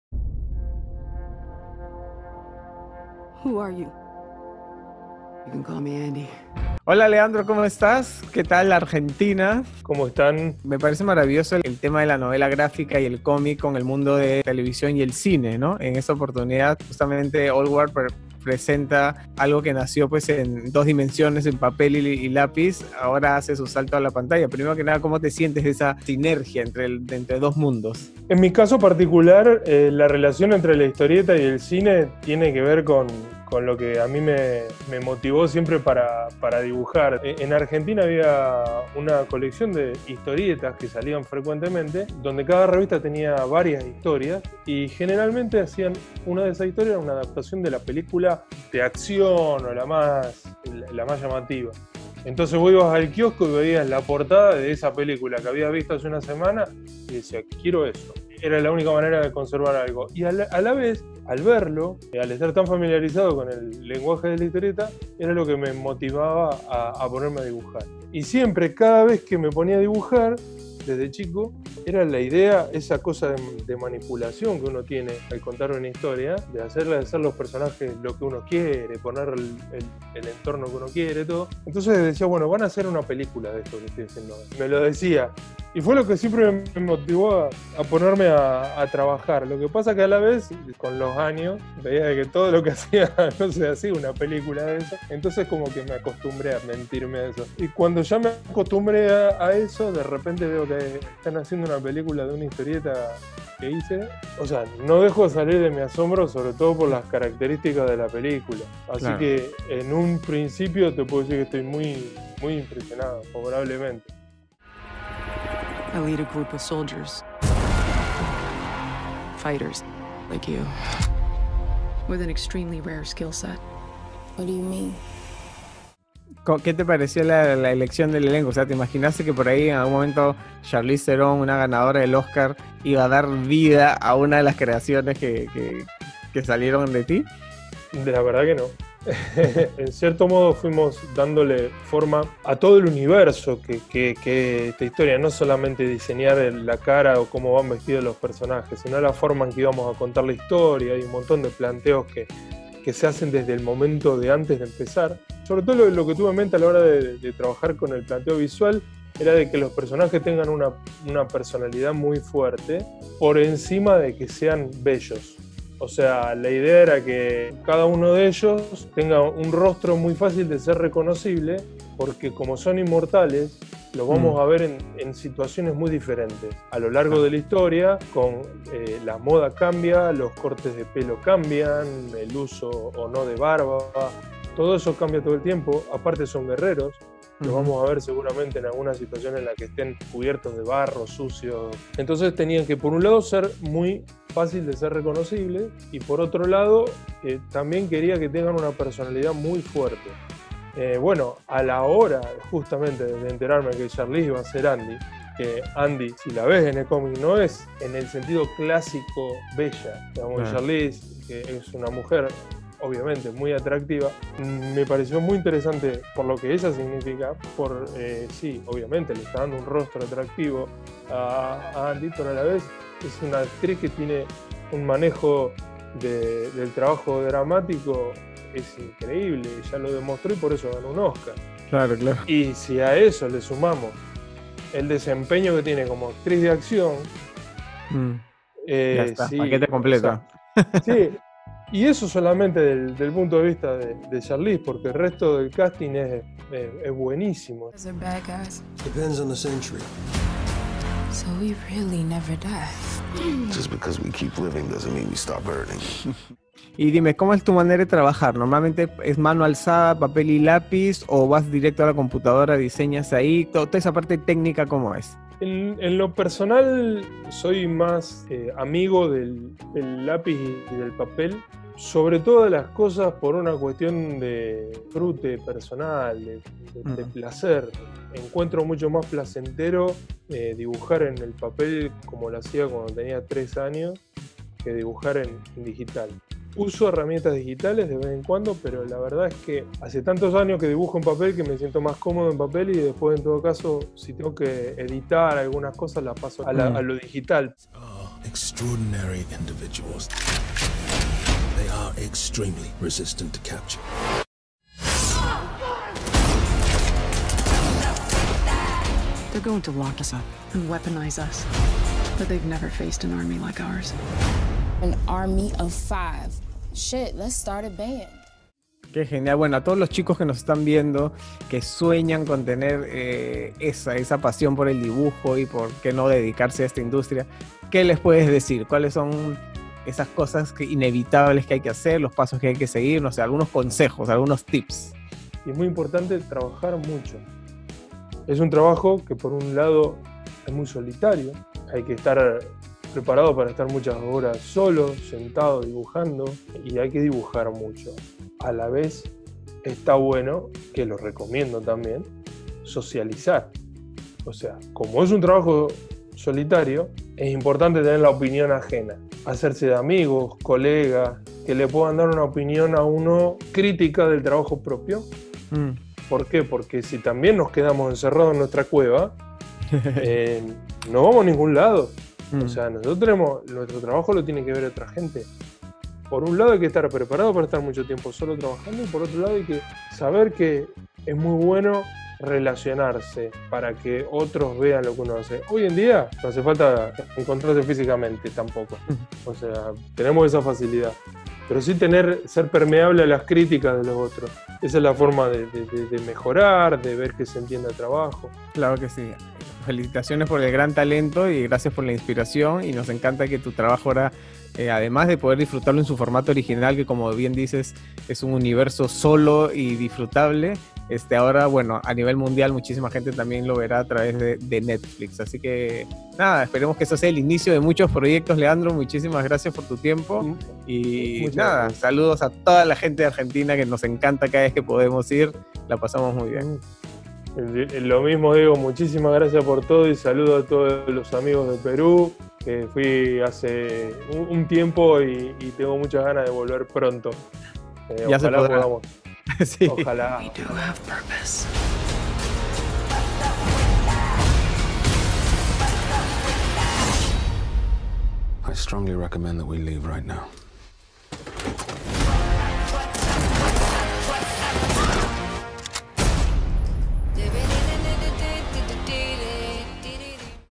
Entrevista: Conoce más de Leandro Fernández, el creador del cómic 'La Vieja Guardia'
'La Vieja Guardia' ya está disponible en Netflix y Bruno pudo conversar con parte del elenco, pero ahora te traemos una nota súper especial pues también conversamos con el argentino Leandro Fernández, quien es el creador del cómic en el que se basó la película ¡No te lo pierdas!